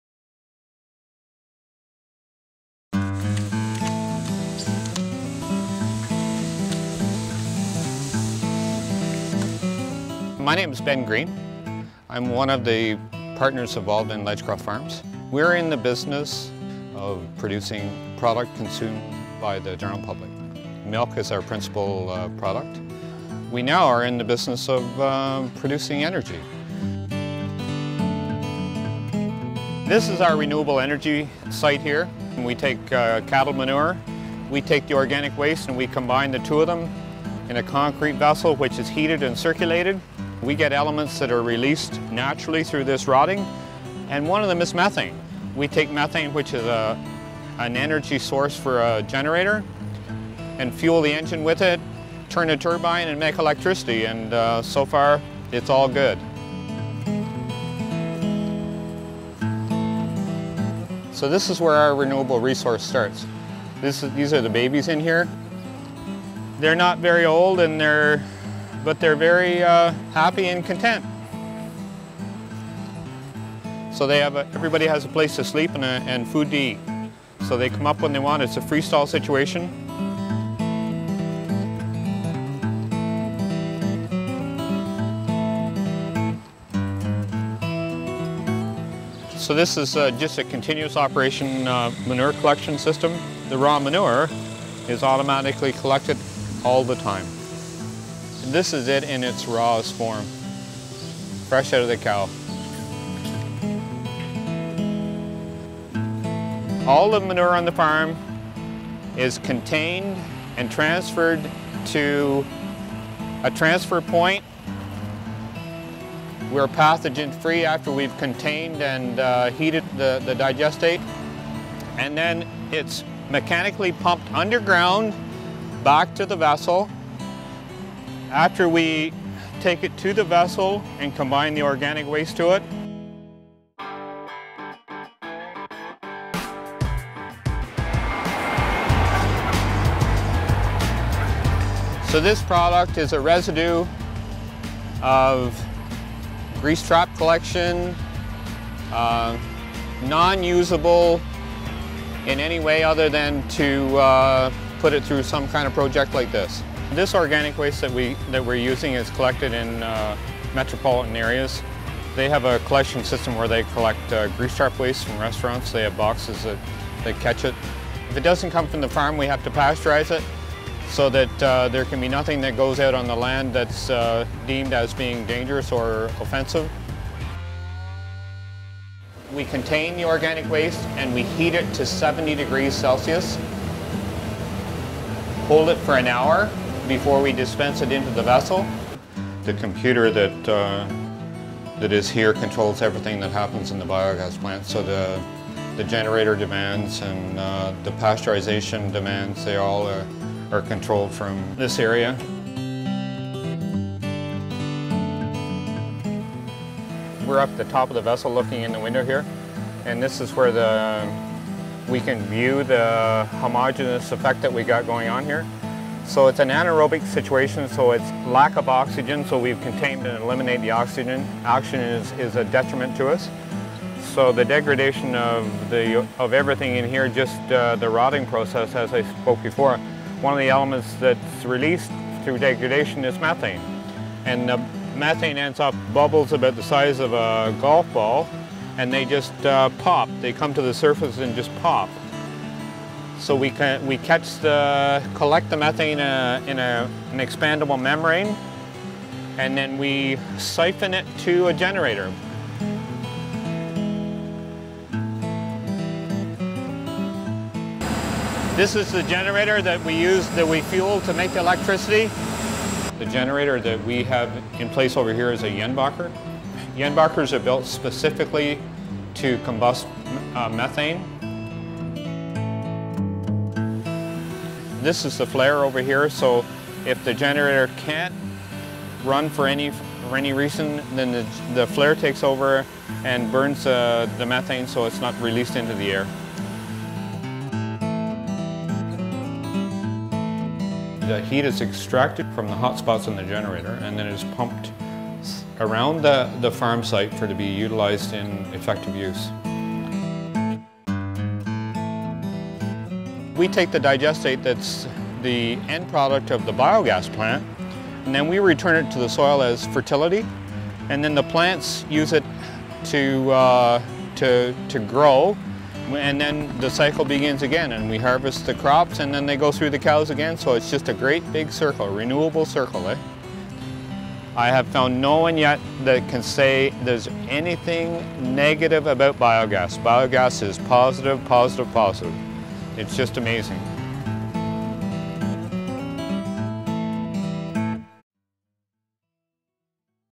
Cow poop for Energy? 2011
Luke made this short documentary for a PhotoSensitive exhibition on alternative energy.
He shot and edited it in one day, and he always liked working alone. This is the last video he made as an independent videographer before getting a regular job close to home.
I think the simplicity of it made it one of his favorite.